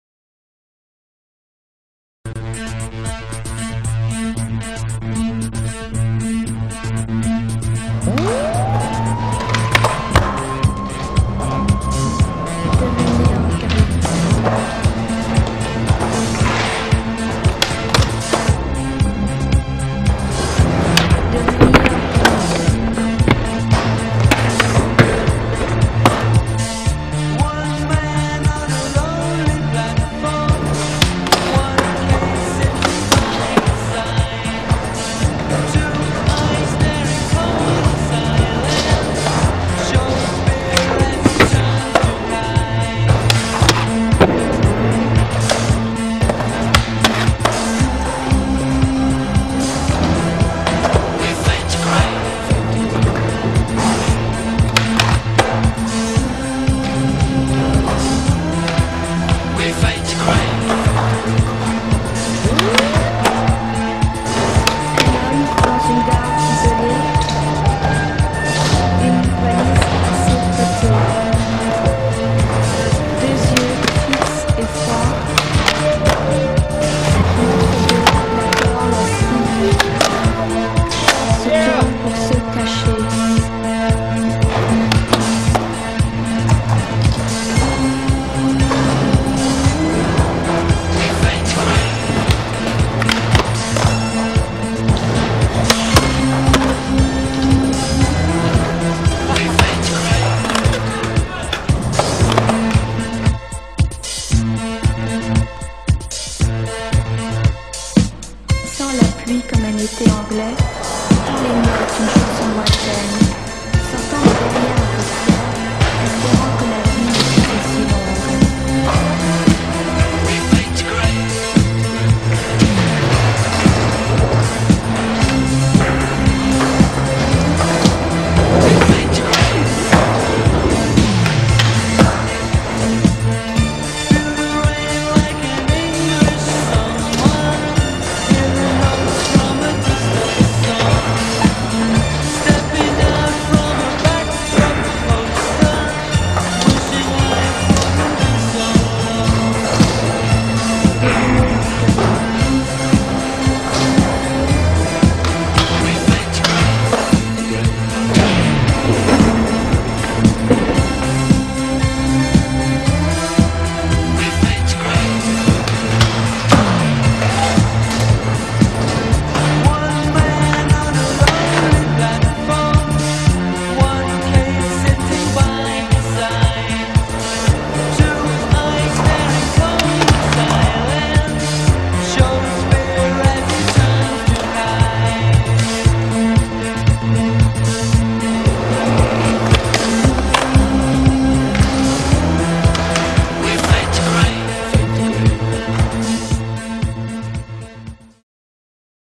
Faith To Grey // Flo Westers, Tarik Ungewitter
Kleiner Zusammenschnitt aus semtlicher Hallenfooty
Farben gefallen mir leider nicht so, was ich hoffentlich durch das Licht entschuldigen darf.